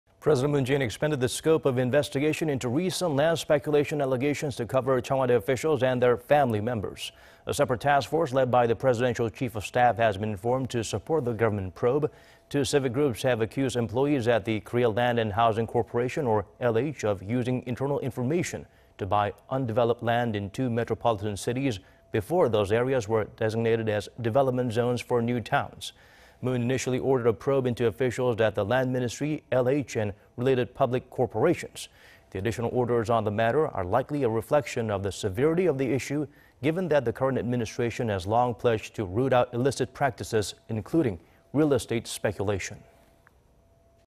Moon orders government probe into land speculation allegations to include Blue House officials
문대통령, 3기 신도시 조사에 靑직원도 포함 지시

President Moon Jae-in expanded the scope of investigation into recent land speculation allegations to cover Cheong Wa Dae officials and their family members.
A separate task force led by the Presidential Chief of Staff has been formed to support the government probe.
Two civic groups have accused employees at the Korea Land and Housing Corporation, or LH, of using internal information to buy undeveloped land in two metropolitan cities... before those areas were designated as development zones for new towns.
Moon initially ordered a probe into officials at the Land Ministry, LH and related public corporations.
The additional orders on the matter are likely a reflection of the severity of the issue,... given that the current administration has long pledged to root out illicit practices,... including real estate speculation.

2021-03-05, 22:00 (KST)